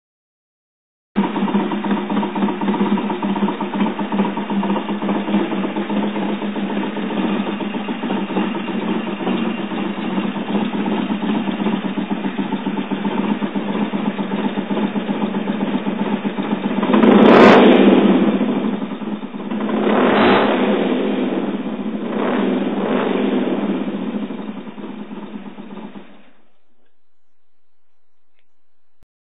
BMW R45 startup 2
Electrics all back in, fuel lines connected and silencers on. Just checking it all works. Carbs not sync'd so a rough idle atm. Probably also need to check the idle mixture screws.